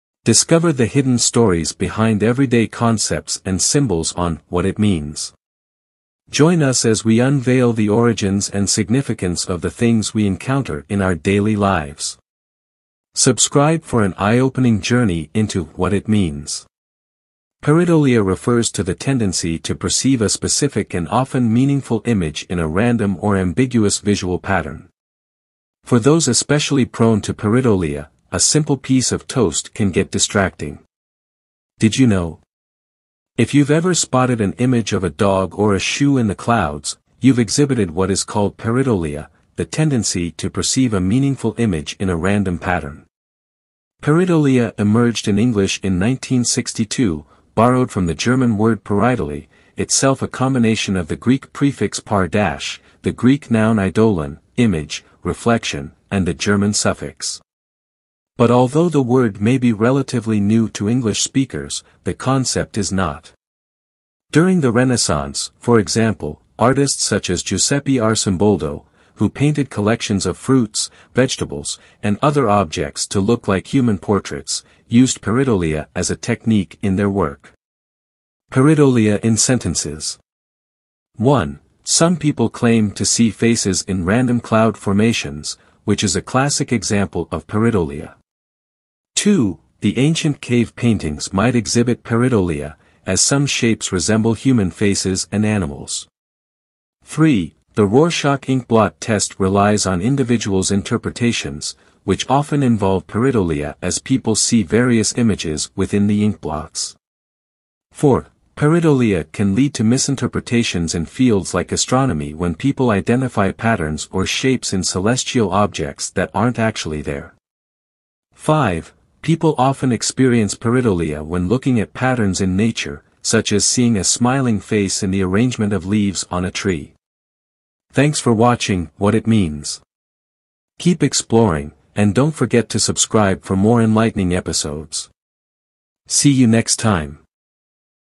What is Pareidolia? Unveiling the Phenomenon What It Means
Professor Christopher here. Welcome to another captivating episode of "What It Means." In this installment, we delve deep into the intriguing phenomenon known as **Pareidolia**.

**Pareidolia** is the fascinating tendency of our minds to perceive specific and often meaningful images in random or ambiguous visual patterns. It's a concept deeply intertwined with **perception**, **meaning in random patterns**, and the **art of interpretation**. Join us as we embark on a journey to explore the hidden stories behind this phenomenon.

Ever found yourself spotting faces in the clouds? That's a classic example of pareidolia at work. We'll show you how **human perception** can conjure up incredible imagery from the most unexpected sources, revealing the artistry in everyday concepts and **natural patterns**.

Unveiling this **phenomenon** means understanding its connection to art and artistic techniques. We'll take a glance back in time at artists like **Giuseppe Arcimboldo**, who used pareidolia as a technique to craft incredible portraits from fruits, vegetables, and other objects.

The **Rorschach Inkblot Test**, reliant on individuals' interpretations, often involves pareidolia, providing insights into the **mind and perception**. We'll also delve into the **history of pareidolia**, showcasing how this concept has been a part of human perception for centuries.

Join us as we discuss the impact of pareidolia on **symbolism**, and the role it plays in visual psychology. This episode is a fascinating exploration of the mind's ability to find meaning in the world's chaos.

Don't miss out on this enlightening journey into the world of pareidolia.